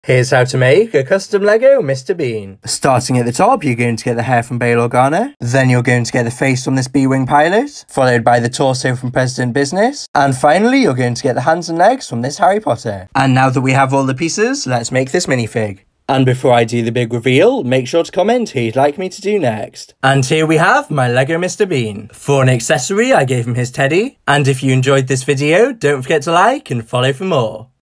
How to make a LEGO Mr Bean Minifigure!
In todays video I show you how to made a custom LEGO Mr Bean minifigure!